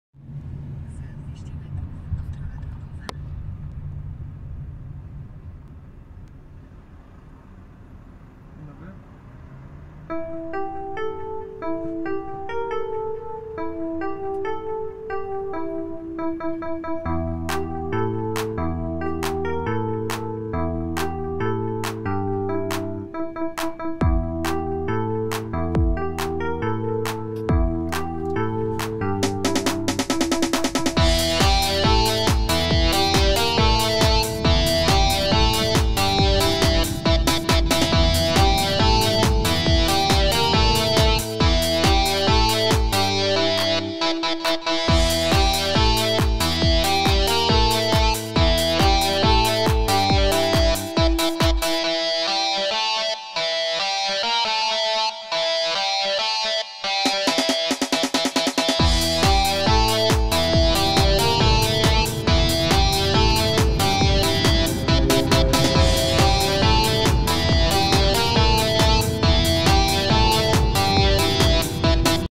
Lamborghini Aventador on highway
Lamborghini Aventador LP 700-4  Caught in Poland  A2 highway / freeway .

Lamborghini Aventador w Polsce na autostradzie A2 kierunek Poznań Warszawa.

Lambo w Polsce na autostradzie - Poznań - Warszawa.